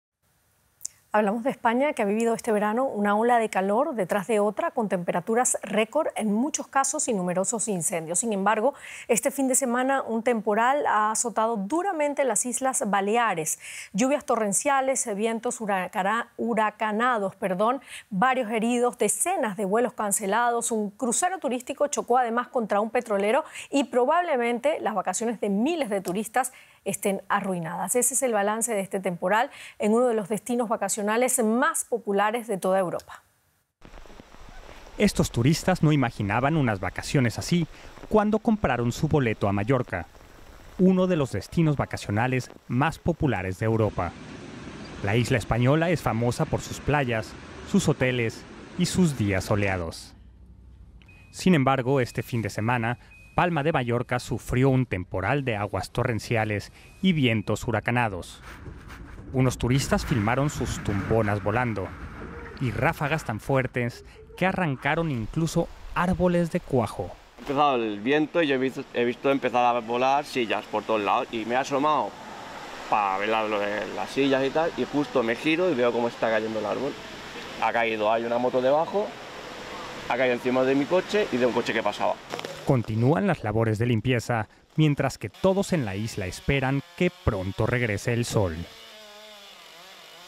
Las lluvias torrenciales provocan el caos en las Baleares
El temporal deja varios heridos, decenas de vuelos cancelados y hasta un choque de barcos en uno de los destinos turísticos más populares de Europa.